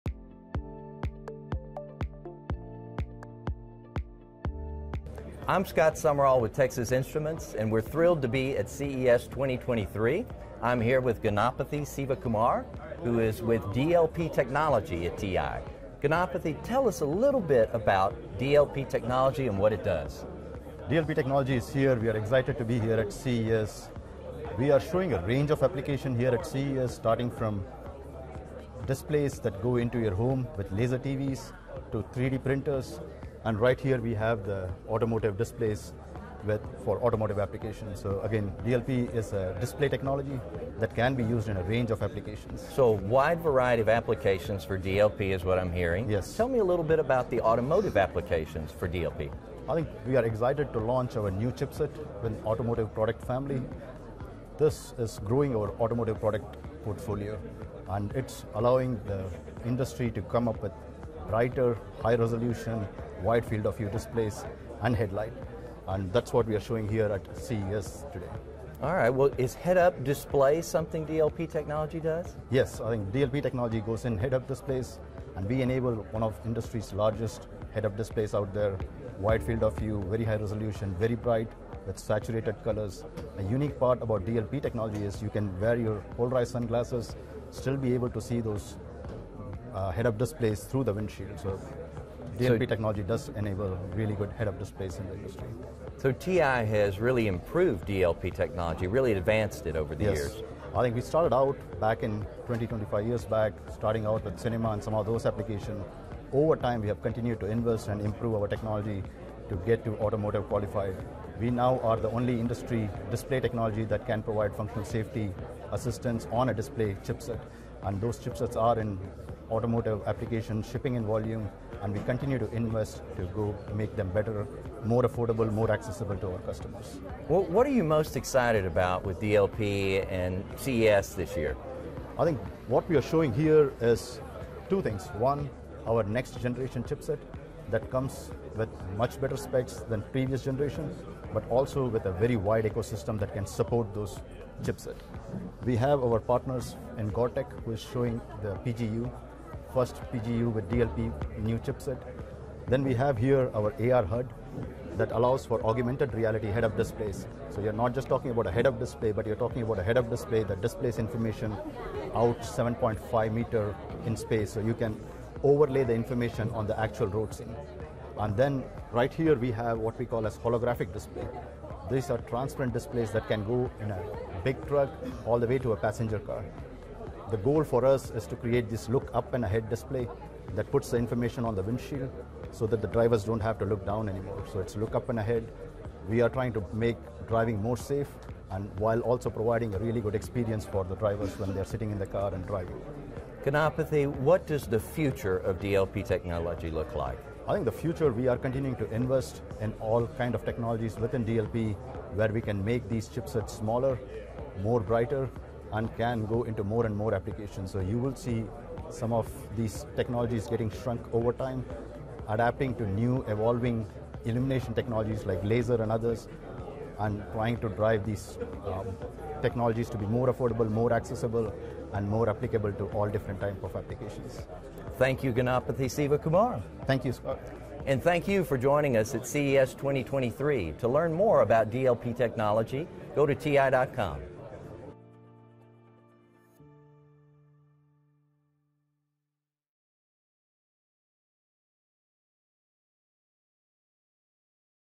Enabling the next generation of AR HUD with high resolution DLP Technology at CES 2023
The DLP4620S-Q1 digital micromirror device (DMD) enables high resolution with a wide field-of-view to project more information to drivers.